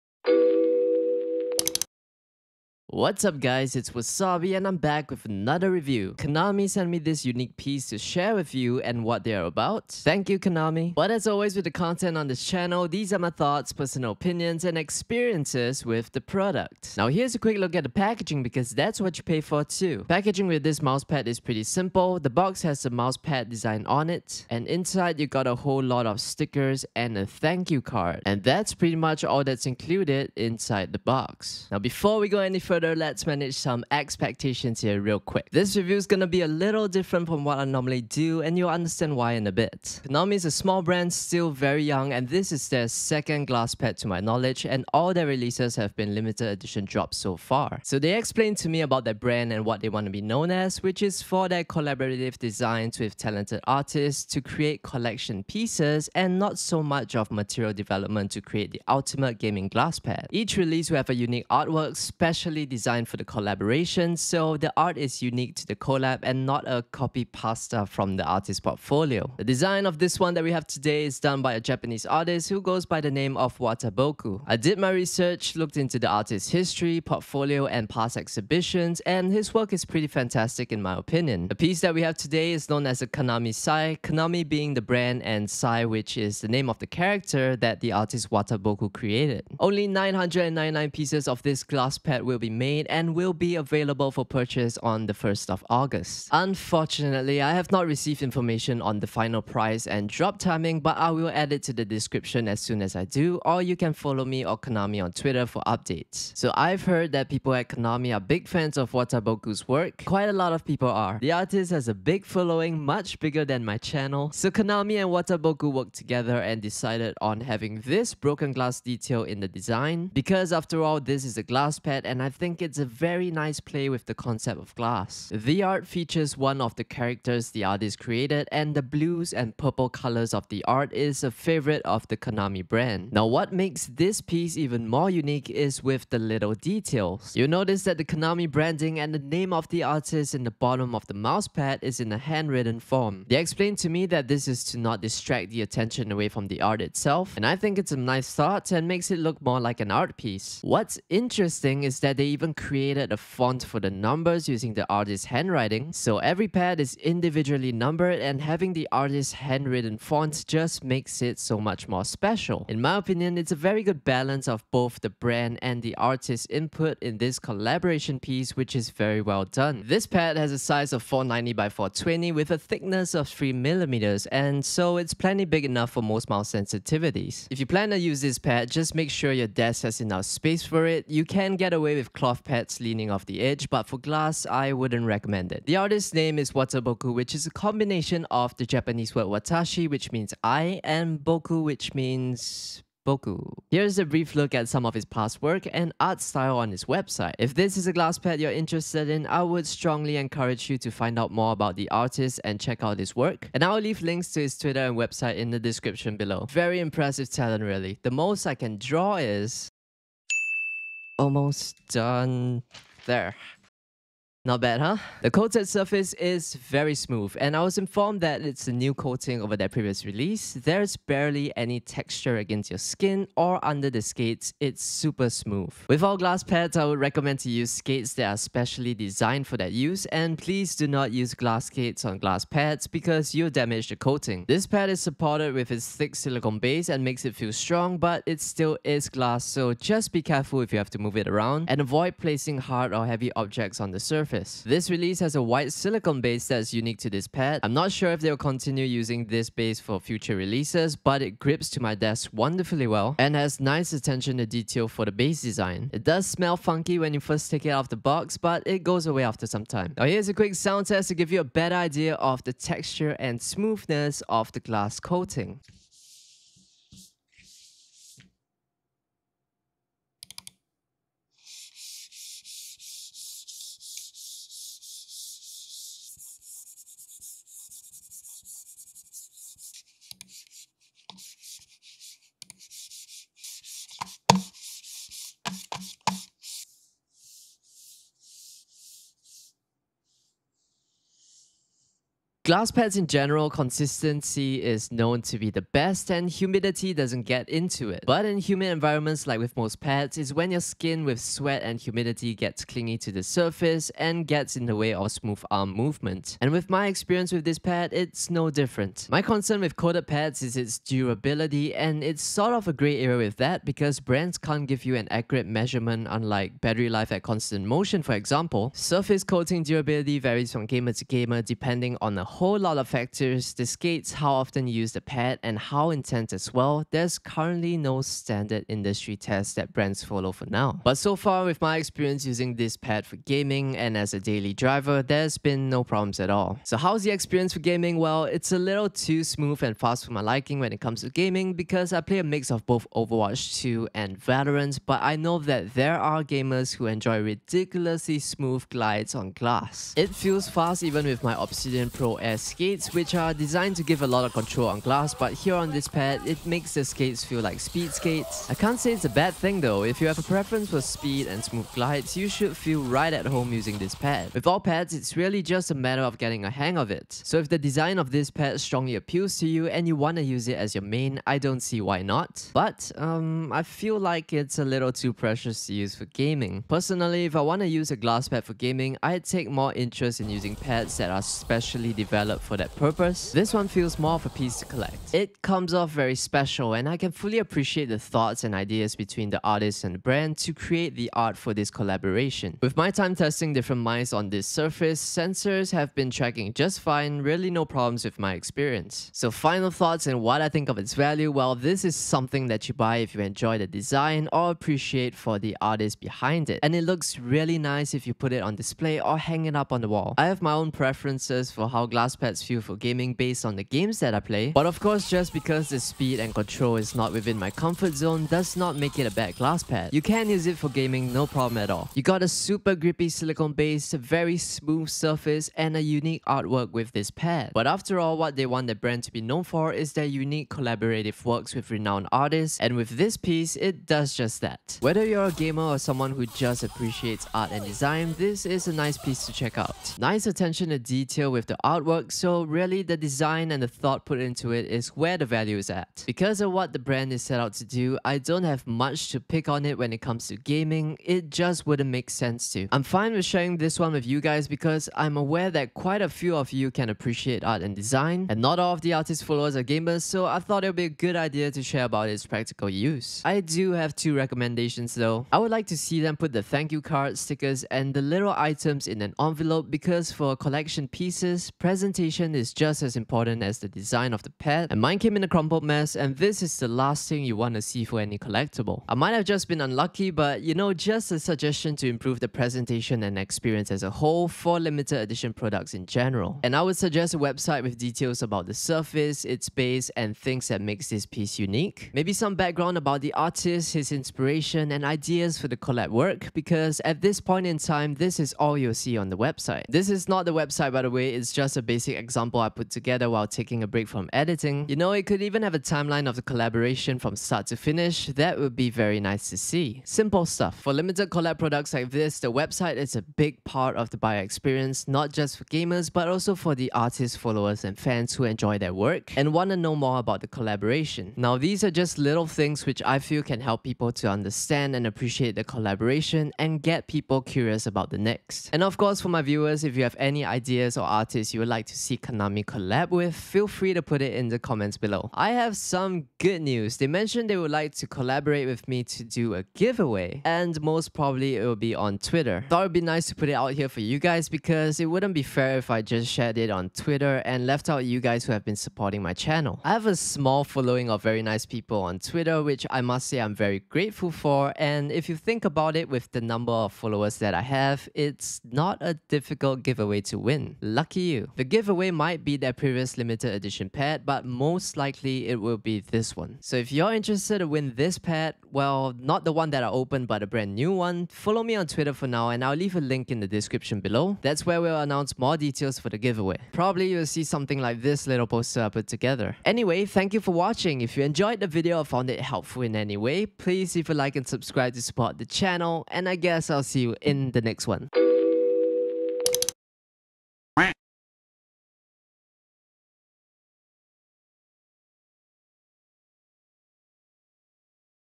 KANAMI x Wataboku Glass Pad Review 1 of 999 + GIVEAWAY! | Before You Buy
A truly unique collector's piece created by a renowned artist (wataboku) using a gaming glass pad as its medium which is rather interesting to see!

In this review, I will touch on details that will help you understand the work behind the pad, its design that makes this piece special, and the experience of using it for gaming.

This review will be a little different from what I normally do as this collaboration involves an artist, so I am taking my audience of gamers and casual consumers and wataboku's fans and admirers of his work into account.

*This is NOT a technical review.
Time Stamps
0:00 Hi Wasabi!
0:16 Unboxing
0:31 Expectations
1:46 Design
2:49 Size
3:05 About - Wataboku
3:35 Material
4:04 Base
4:25 Sound Test
4:52 Humidity & Consistency
5:36 Experience
6:32 Sensor Compatibility
6:39 Final Thoughts
7:26 Recommendations
8:55 GIVEAWAY

Case: Lian-li O11 DYNAMIC
CPU: AMD Ryzen 7 5800X 8-Core
RAM: XPG DDR4 D50 RGB 8GB 3600MHz x2
MOBO: Gigabyte B550 VISION D-P
GPU: GIGABYTE Vision GeForce RTX 3080